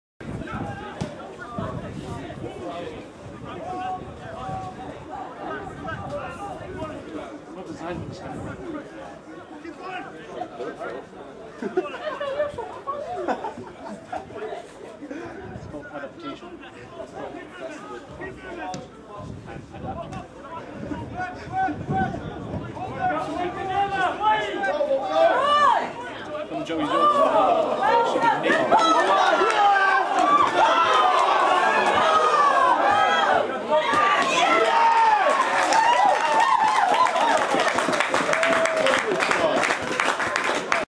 England Colleges Rob Allen Try
Rob Allen's (14) Try against Scotland Universities Lasswade RFC, Edinburgh, Scotland Friday 27th February 2009